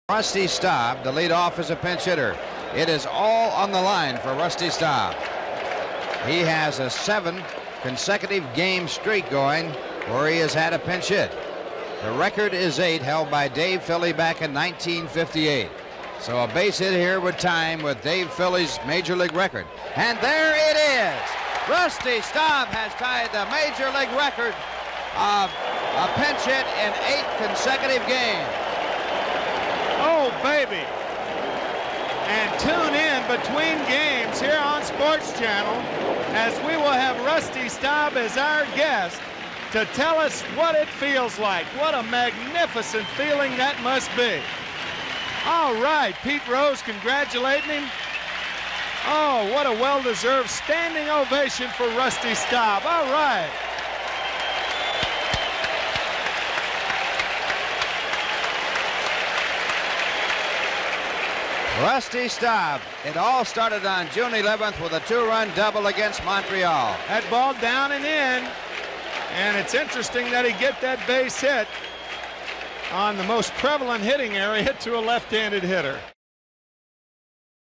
Staub's consecutive pinch-hit streak reaches eight
6/26/83: Rusty Staub records a single for his eighth consecutive pinch-hit appearance with a hit